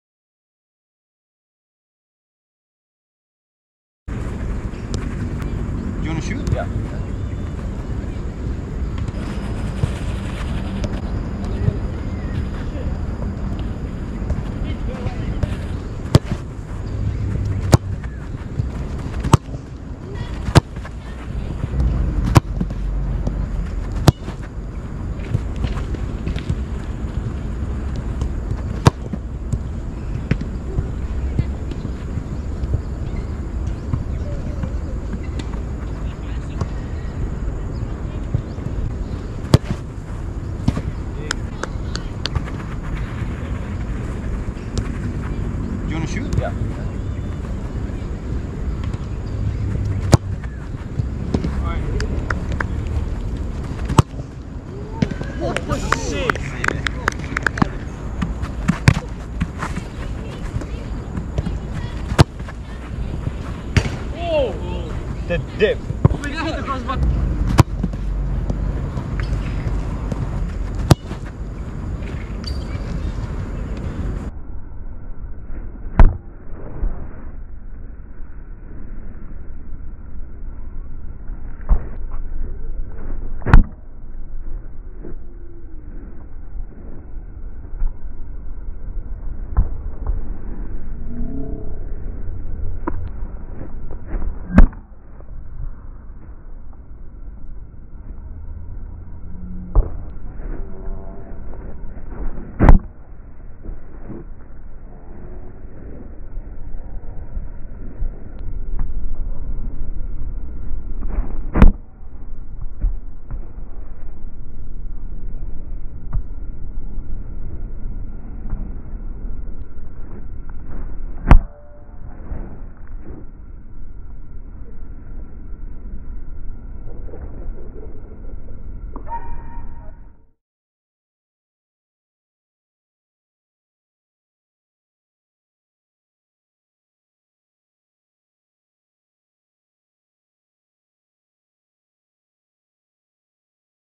Puma Future Z vs Nike Mercurial Superfly
Football Boot VS: on artificial grass (AG) pitch.
Puma Future Z 3.2 FG/AG vs Nike Mercurial Superfly 7 Academy MG review